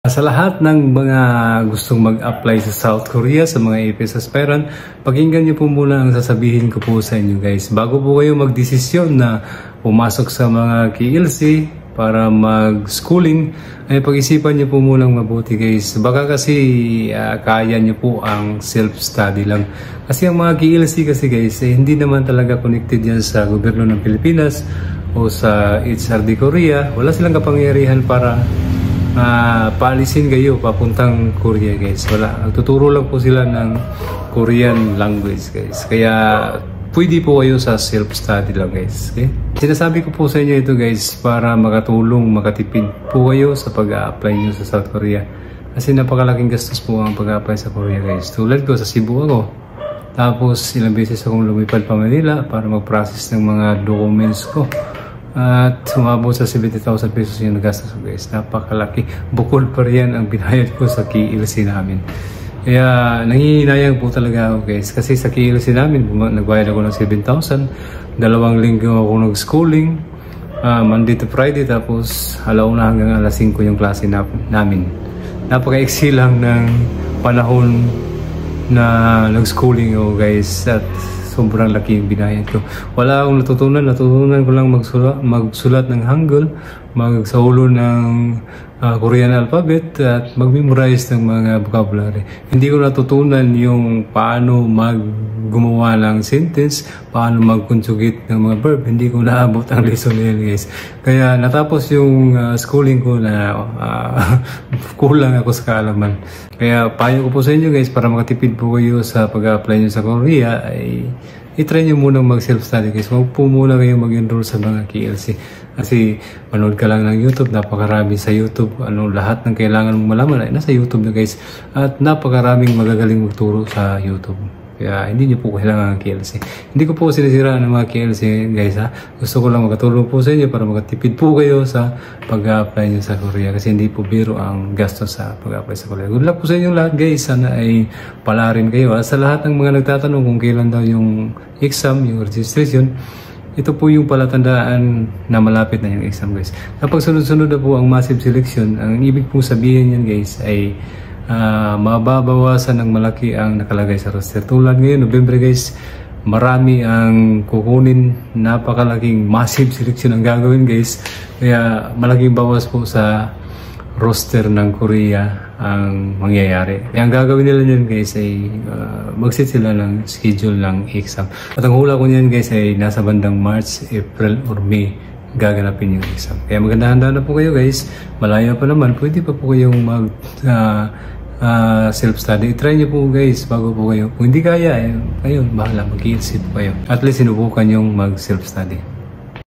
Kailan ang EPS TOPIK EXAM? Wag muna kayo mag KLC
Sa video na ito ituturo ko po sa inyo paano makatipid sa pag aaply ng trabaho sa south korea.Bago po kayo mag desisyon mag enroll sa mga KLC ay dapat subukan nyo muna mag self study.